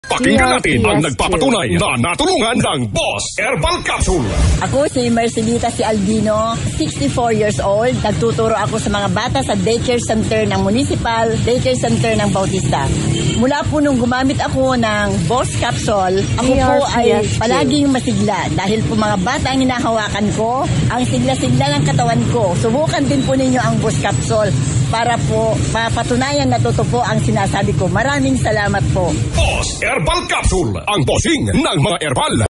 Bos Herbal Capsule Testimonial Radio Ad 2023 36s (Version 4)
Ito po ang 2023 Testimonial Radio Ad ng Bos Herbal Capsule. (Version 4)

DRPS Two 5365: Bos Herbal Capsule Testimonial Radio Ad 2023 36s (Version 4)